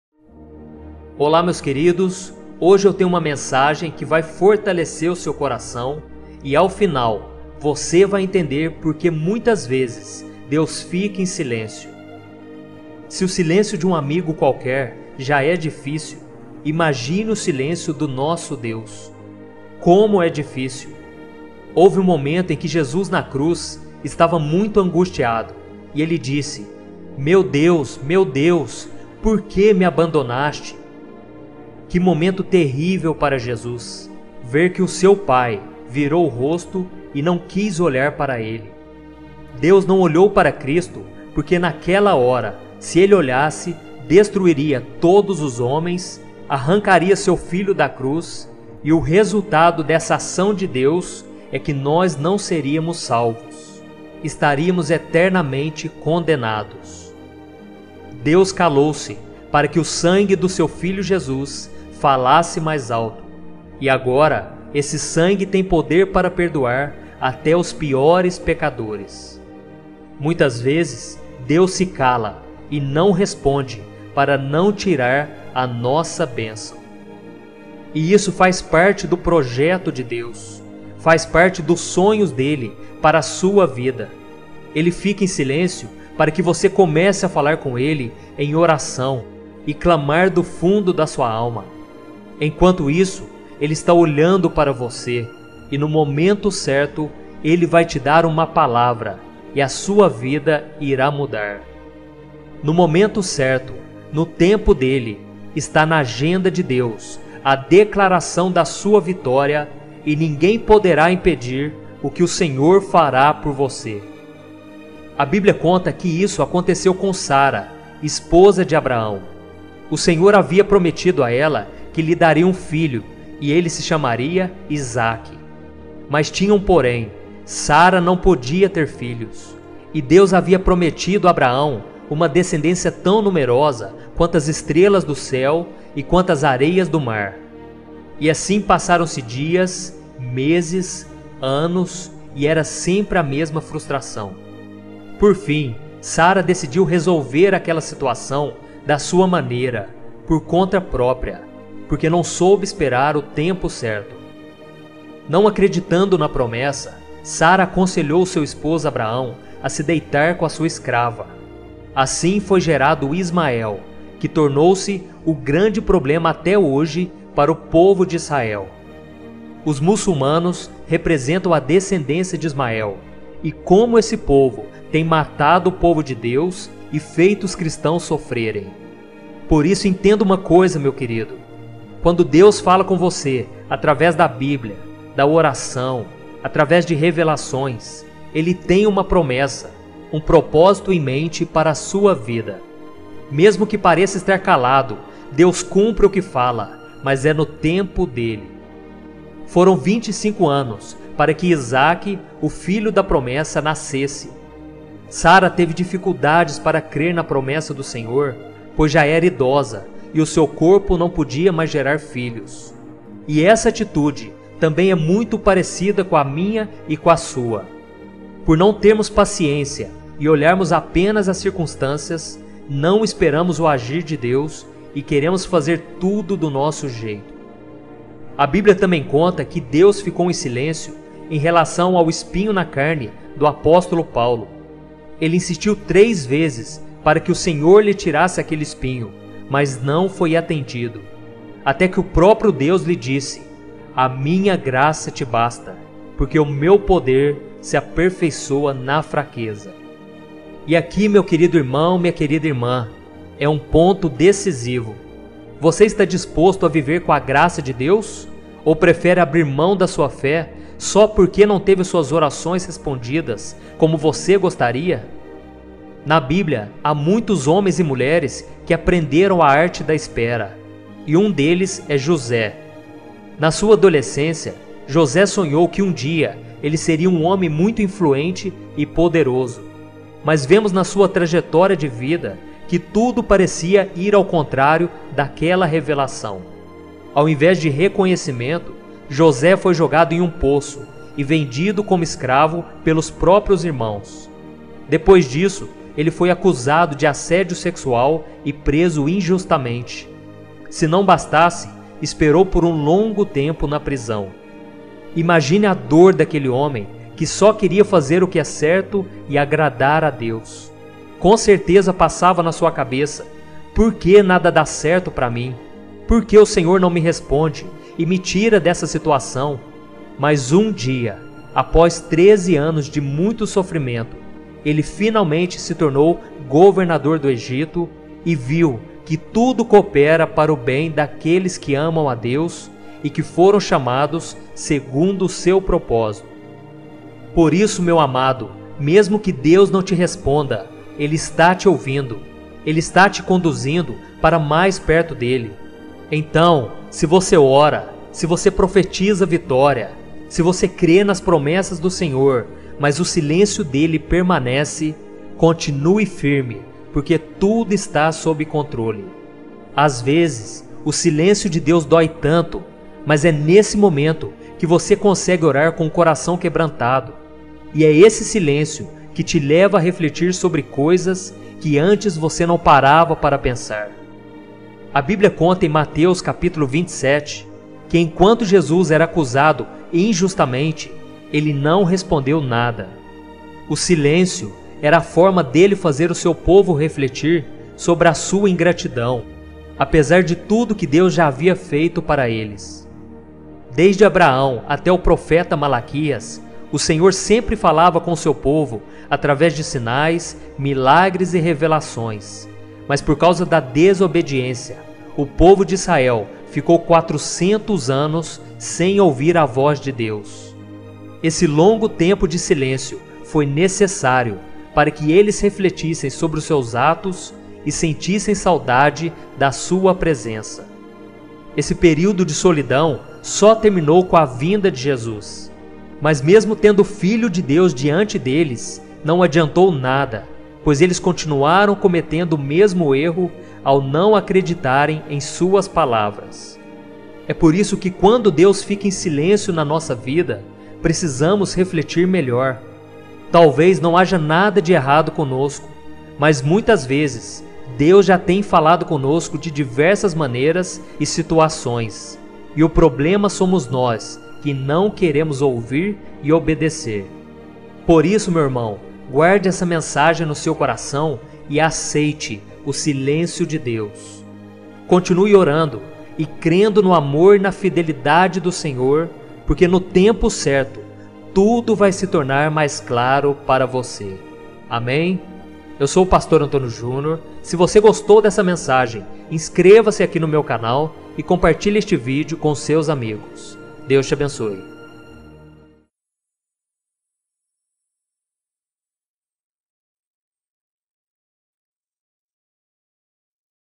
VOCÊ NÃO CONSEGUE OUVIR A VOZ DE DEUS? - Saiba Porque Deus Fica em Silêncio
Se você quer ouvir a voz de Deus e tem esperado uma resposta, ouça esta mensagem até o final. Muitas vezes Deus fica em silêncio e é muito importante que você saiba porque Ele faz isso.

Quer receber uma mensagem de Deus todos os dias? Aqui também você ouvirá pregação da Palavra de Deus para fortalecer sua fé!